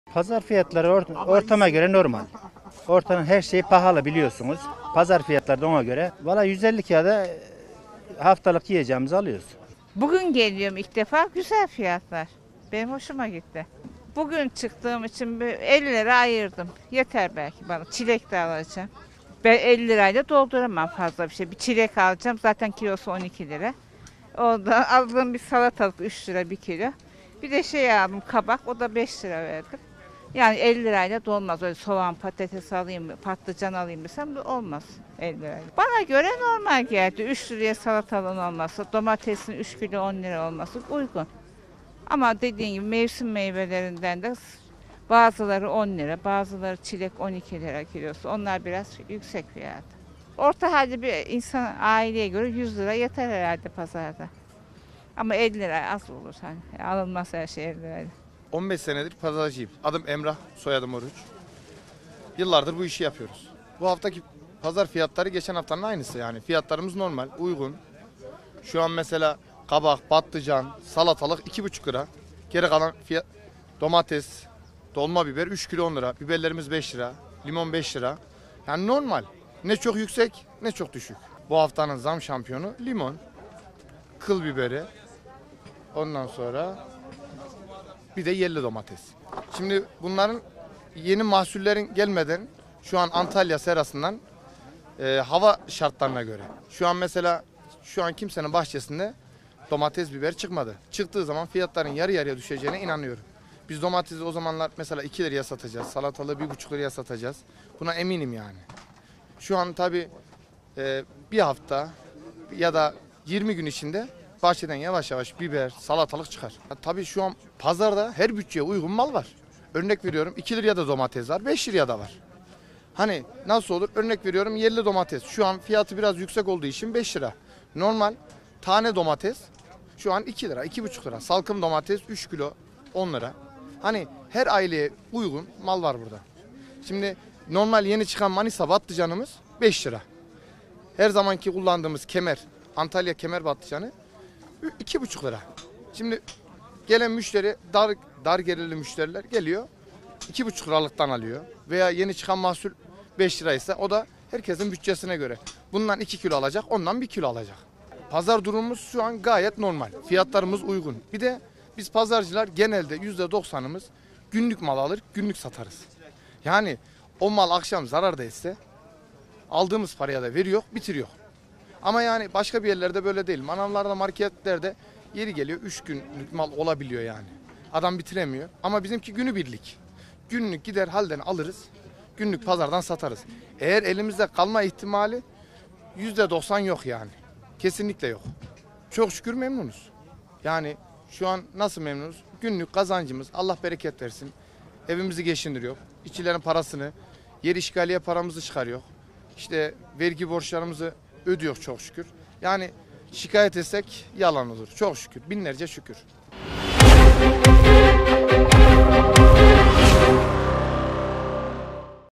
Pazar Tezgahları Seralara Göz Dikti
KAYSERİ,(BGC)-Yaz mevsiminin ortalarına doğru pazar yerlerinde hareketlilik giderek artıyor. Tezgâhtarlar ve müşteriler pazar fiyatlarının yaz mevsimine göre normal seyirde gittiğini vurgularken, müşteriler fiyatların halen pahalı olduğunu belirtiyor. Ayrıca pazarcılar seralardan gelecek ürünleri beklediğini söylüyor.

Gelecek haftadan itibaren Pazar günleri sokağa çıkma kısıtlamasının kaldırılması bekleniyorken, pazarlarda hareketlilik giderek artıyor. Pazarcılar fiyatların yaz mevsimine göre makul seviyelerde seyrettiğinin altını çiziyor. 15 yıldır pazarcılık yapan Emrah Oruç, fiyatlarının uygun olduğunu belirterek “Kabak, patlıcan, salatalığın 2,5 lira; domates, dolma biberin 3 kilosu 10 TL” diye konuştu.

Bu haftanın zam şampiyonu olan ürünlerinin limon, kıl biberi ve yerli domates olduğunu söyleyen Oruç, “Domates ve biber çıktığında fiyatlar yarı yarıya düşecektir. Antalya seralarından hava şartlarına göre 20 gün içinde biber ve salatalık çıkar” diye konuştu. Oruç, her bütçeye uygun mal olduğunu belirtti. Ürünlerin türlerine göre de fiyat farklılığı olduğuna dikkat çeken Oruç, “Örneğin yerli domatesin kilosu 5 TL iken, tane domates 2,5 TL, salkım domatesin 3 kilosu 10 TL. Manisa patlıcanı 5 TL iken, Kemer patlıcanı 2,5 TL’den satılıyor” dedi.

Günübirlik alım ve satım yaptıklarını, stok mal satmadıklarını ve manavlarda dahi 2-3 günlük mal bulunurken kendilerinin günlük ürün sattığının altını çizen Oruç, “Mallarımızın elimizde kalma ihtimali yok denecek kadar az” dedi.

Pazara alışverişe gelen vatandaşlar ise fiyatların pahalı olduğunu belirtti. Bütçesinin 50 TL olduğunu belirten vatandaş “Bana göre sebzelerde fiyatlar normal. Mevsim meyvelerinin fiyatları ise normal değil” diye kanaatte bulunurken, bütçesinin haftalık 150 TL’yi bulduğunu söyleyen bir başka vatandaş ise her şeyin pahalı olduğunu belirterek “Buraya gelene kadar fiyat ikiye, üçe katlıyor” diye konuştu.